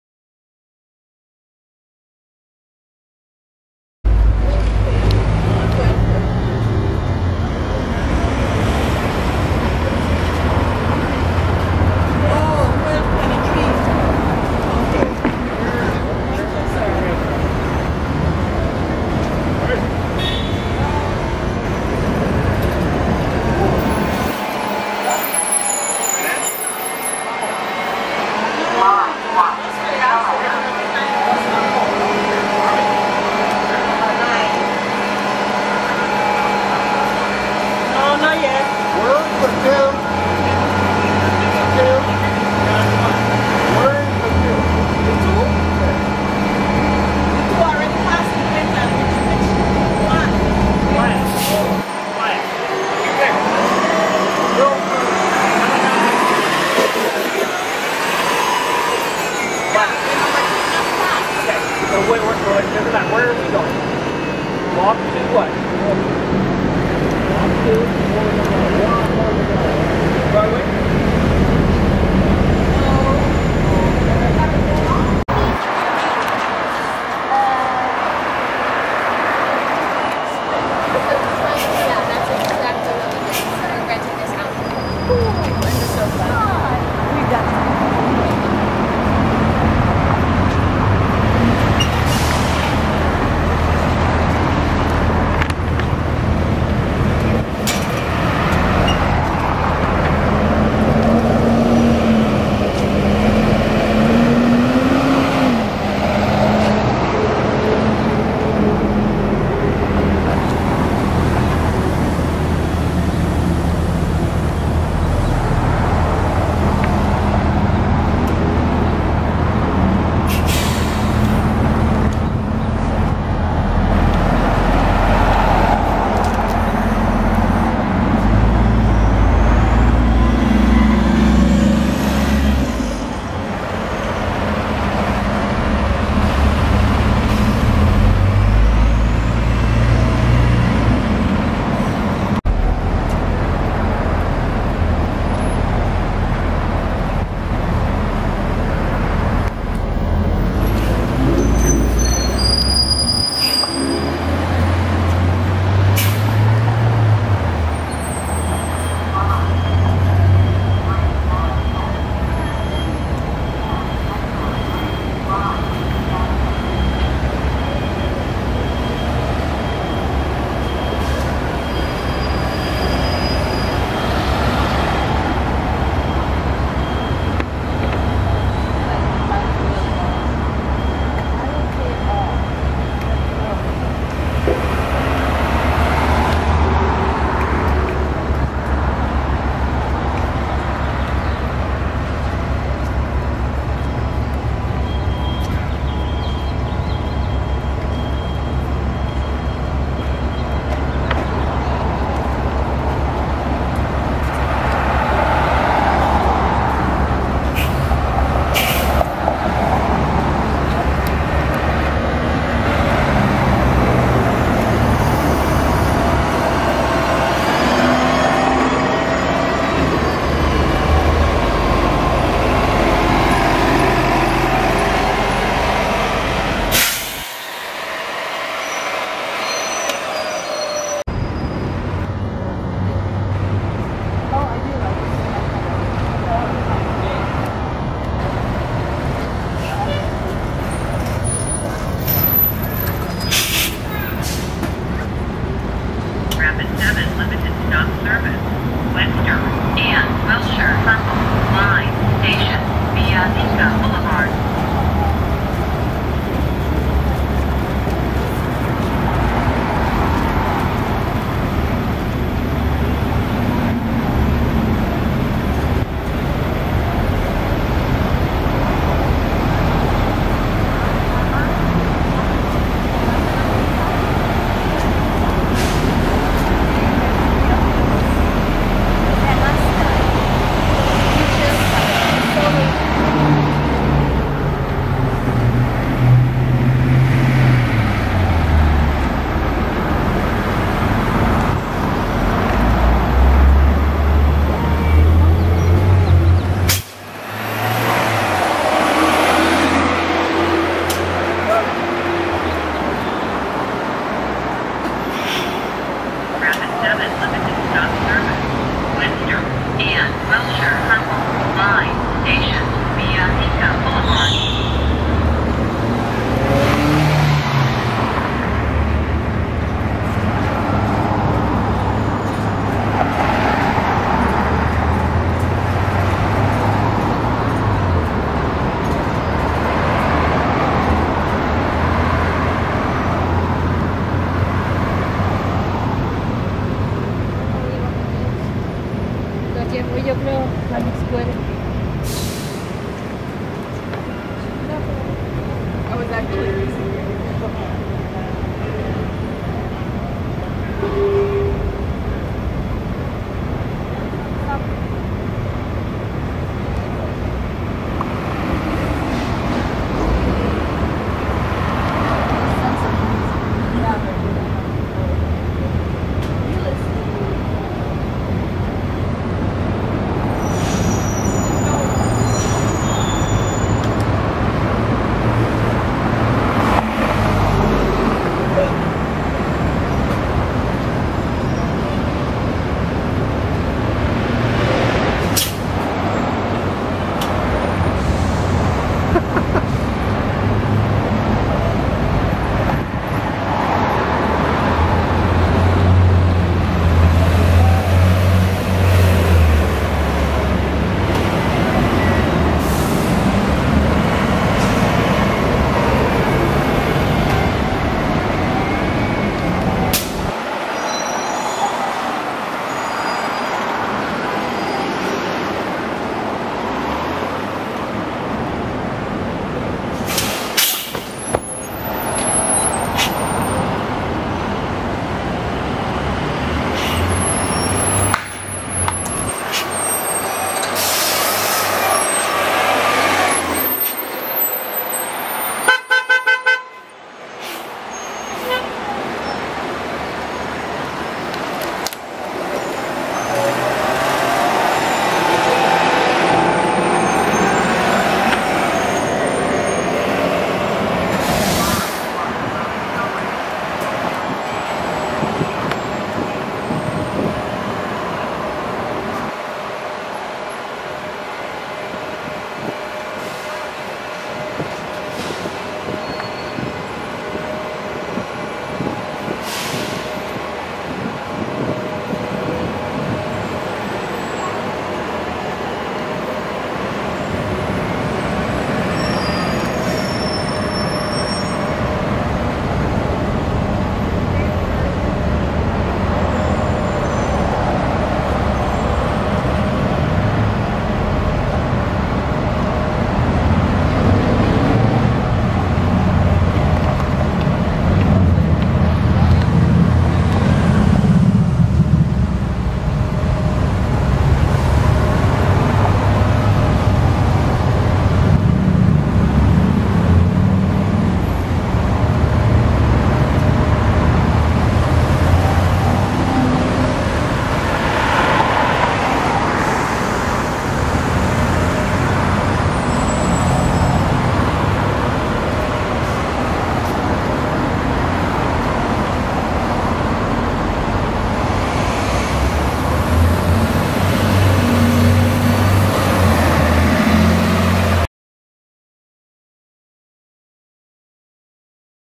6/29/2016 Santa Monica Big Blue Bus
Spent some time in Santa Monica today to take photos of the Big Blue Bus and do some videos hope you like what I came up with.